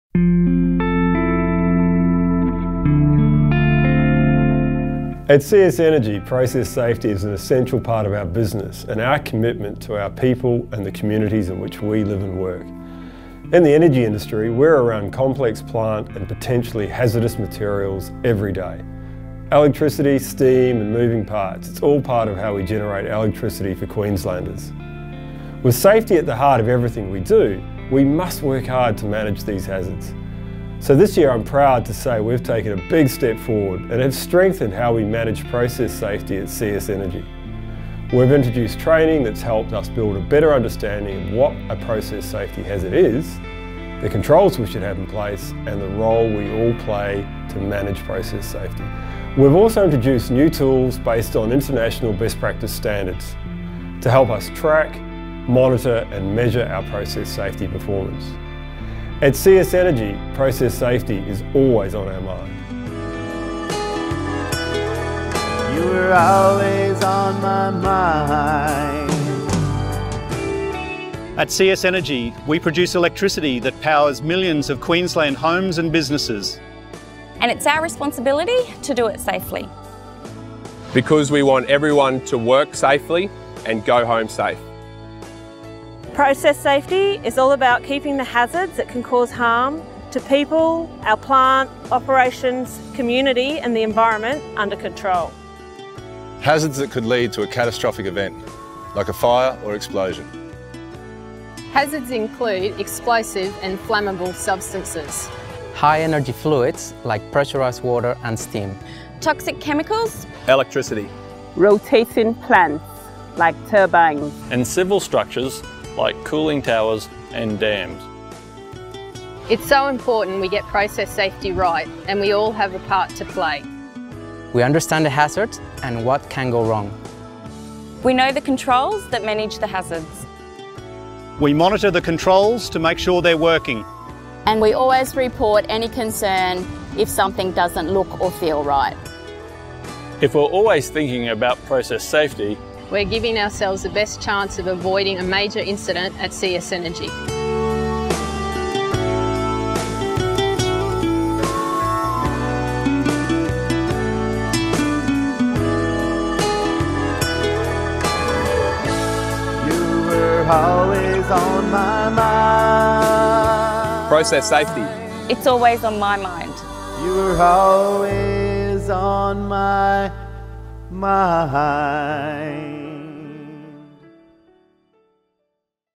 Process Safety at CS Energy
Learn how we're keeping Process Safety always on our mind at CS Energy.